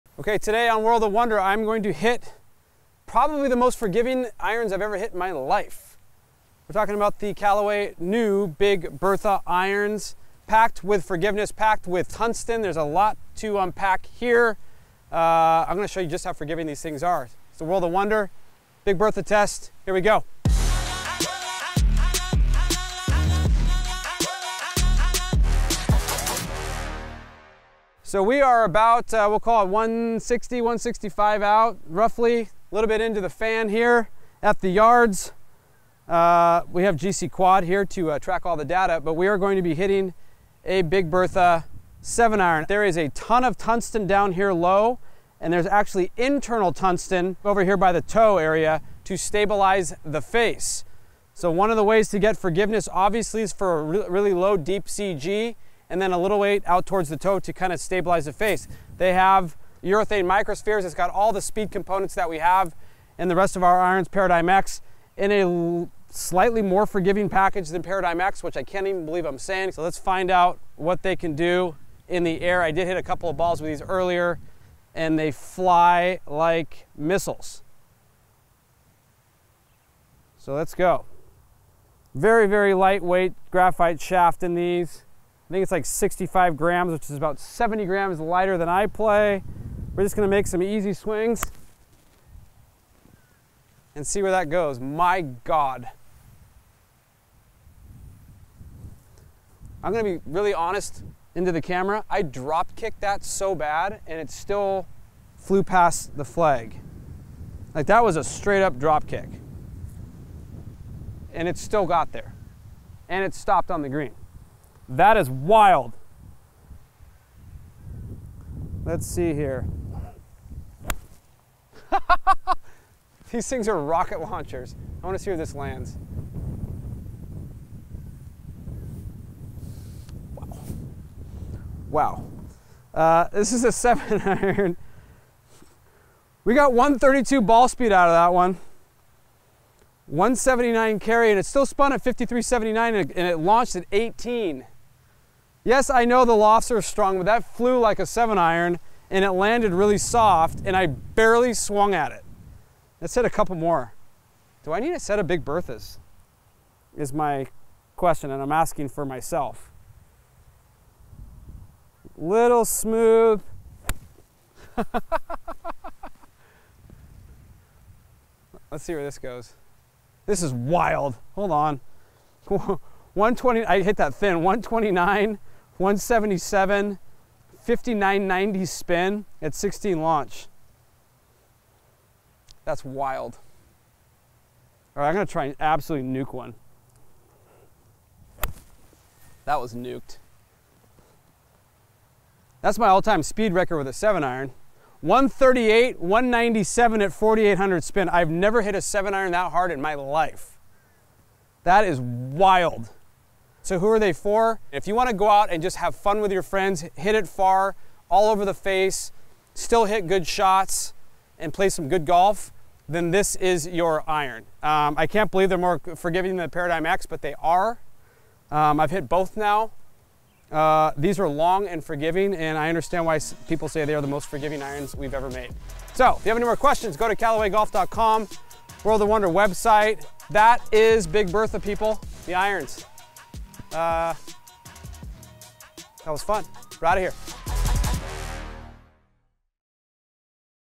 [캘러웨이] 빅버사 아이언 시타 | 강력한 비거리와 빠른 스피드 구현
정밀 텅스텐 웨이팅 기술과 최적의 런치앵글과 컨트롤로 높은 관용성을 제공하는 '빅버사' 아이언